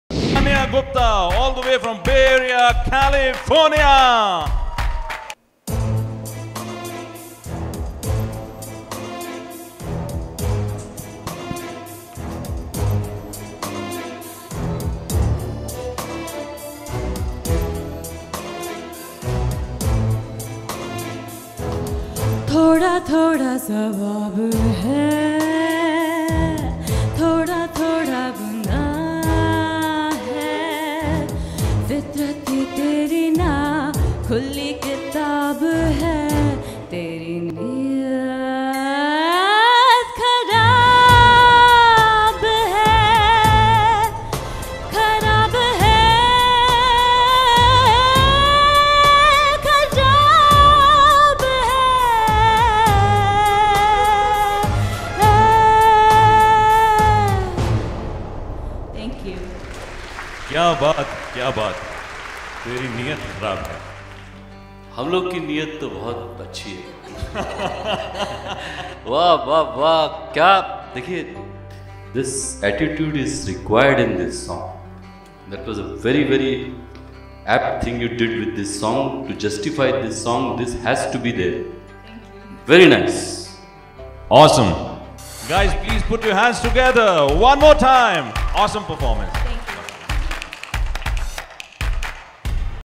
INTERNATIONAL INDIAN ICON - 3iii 2019 Season-III
3iii INTERNATIONAL INDIAN ICON - 3iii 2019 Season-III
 A Platform for Indian Talent Across the Globe!
3iii 3235 - Tanya Gupta-Sr Solo Singing- Gee Vision Inc.